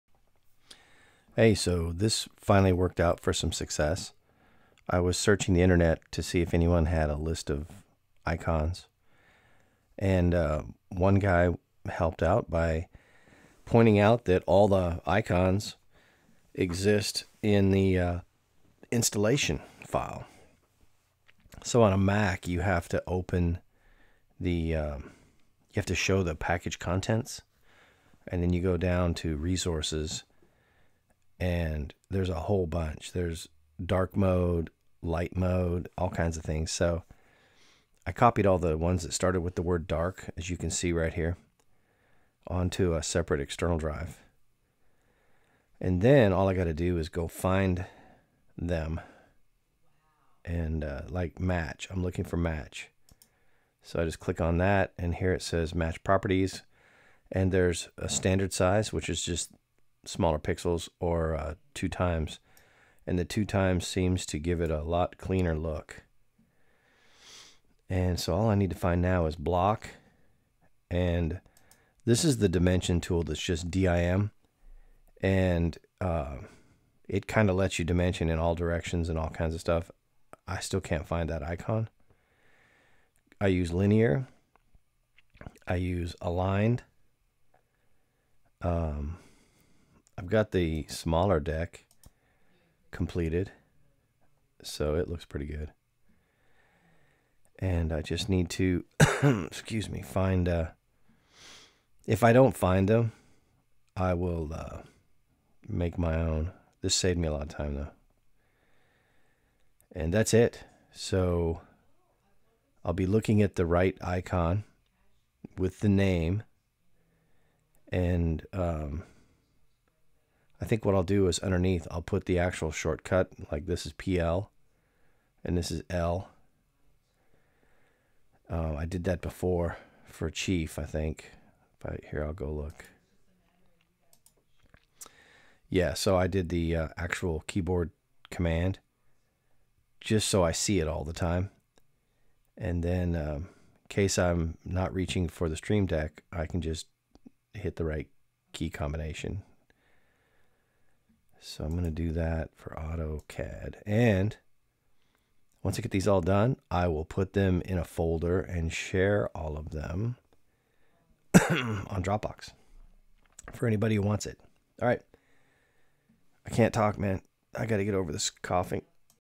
AutoCAD Icons
Who knew?
So the icons that I'm looking for to install in my stream deck, so that they match the tool that I'm using, are actually located inside the autoCAD program itself.